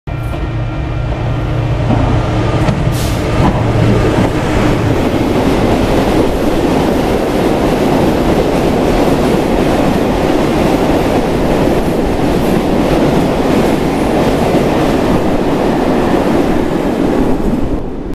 M Int 53180 {siderúrgicos} Valença-Penalva (Devesas - 22.11.13)
Passagem noturna em força deste comboio com caixas móveis da Tramesa Steel a reboque da LD Vossloh Euro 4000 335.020